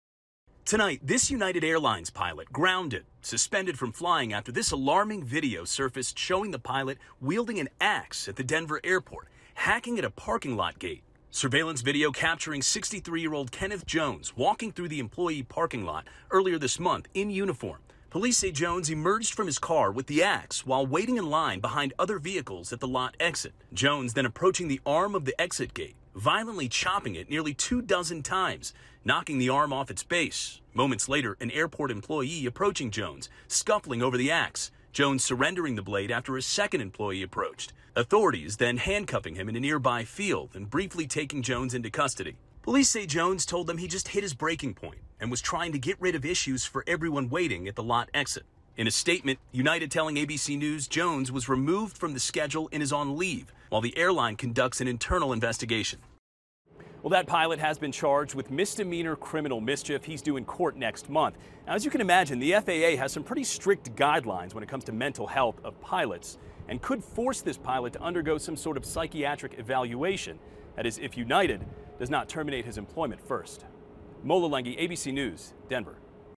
United pilot charged in ax attack in Denver airport parking lot
A United Airlines pilot went on an ax attack, chopping a parking arm gate off, in a Denver airport employee parking lot. Police say Kenneth Henderson Jones told them "He just hit his breaking point." FULL STORY